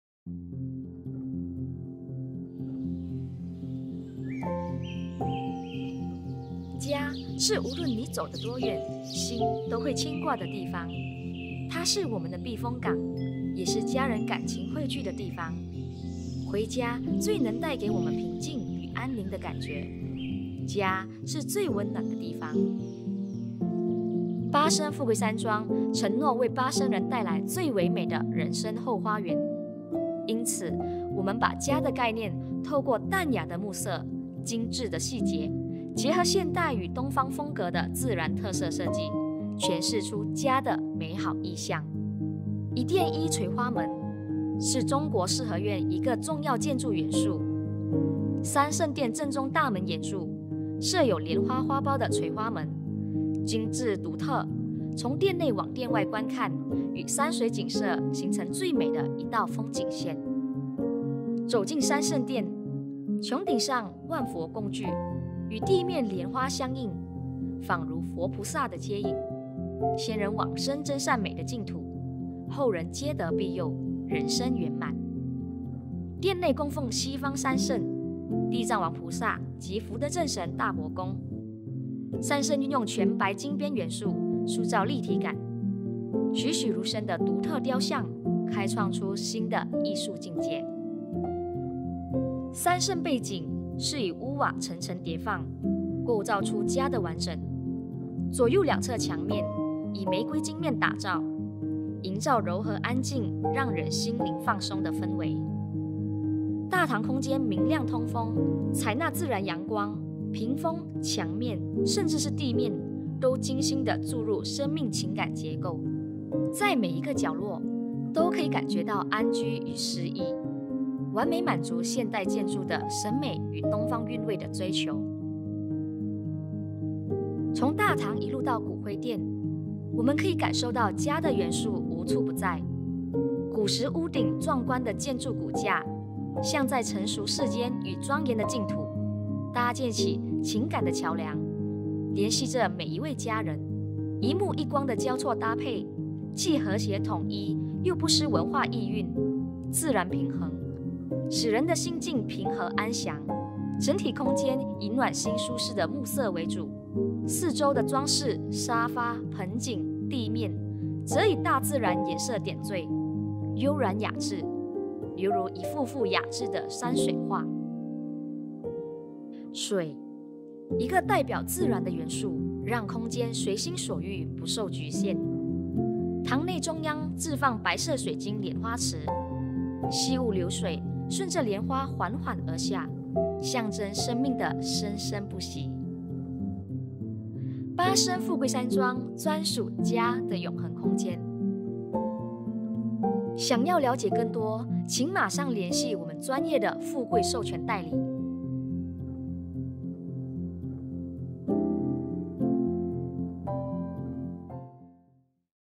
The eternal home beyond comparison - Nirvana Memorial Park (Klang) 专属“家”的永恒空间 - 富贵山庄（巴生）
Nirvana Memorial Park (Klang) promises to bring to the people of Klang the most beautiful memorial park. Therefore, We have placed the "concept of home" through elegant wood tones, exquisite details, and natural element design that combines modern and traditional oriental styles to interpret the beautiful visual of "home".

富贵山庄（巴生） 承诺为巴生人带来 最唯美的人生后花园 因此 我们把“家的概念” 透过 淡雅的木色 精致的细节 结合现代与东方风格的自然特色设计 诠释出“家”的美好意象